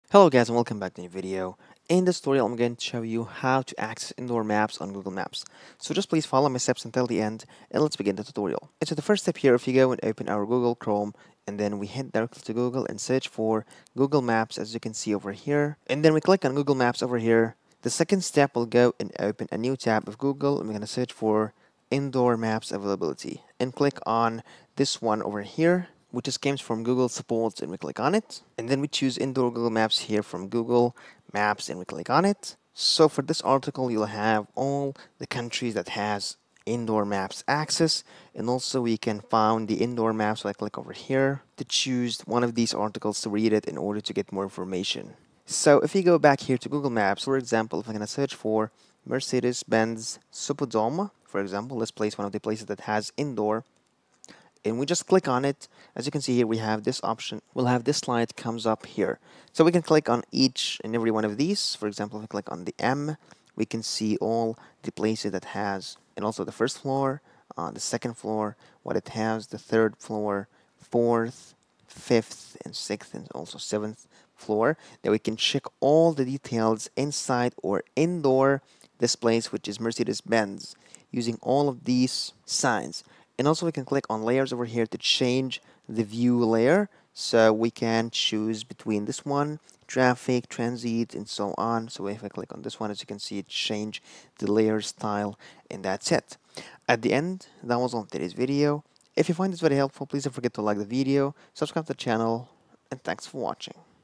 How to Use Indoor Navigation in Google Maps (2024)
Discover how to use indoor navigation in Google Maps with this 2024 tutorial! Whether you're navigating through a large shopping mall, airport, or other complex indoor venues, this guide will show you how to use Google Maps' indoor navigation feature to find your way efficiently. Learn how to access indoor maps, locate points of interest, and navigate indoors with ease.